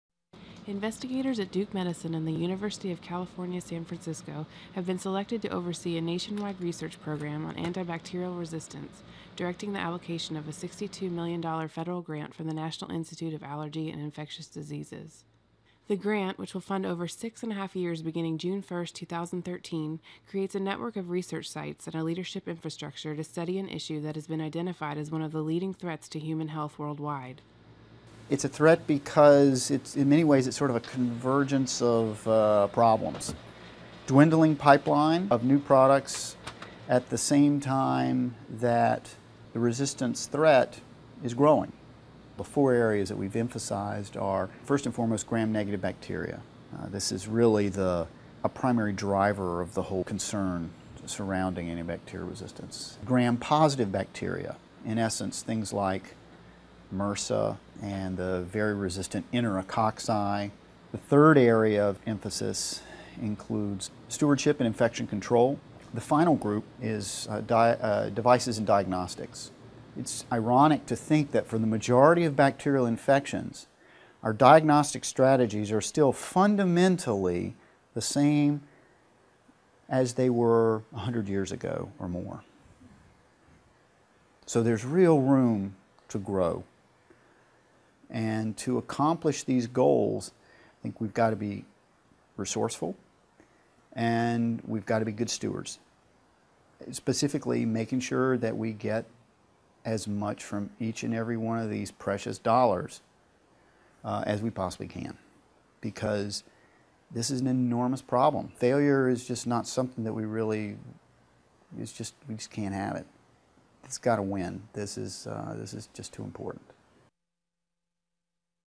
Duke receives $62 million grant to lead network for clinical research on antibacterial resistance
Investigators at Duke Medicine and the University of California San Francisco have been selected to oversee a nationwide research program on antibacterial resistance, directing the allocation of a $62 million federal grant from the National Institute of Allergy and Infectious Diseases. Vance Fowler, Jr., M.D., MHS, professor of medicine at Duke is co-principal investigator of the network.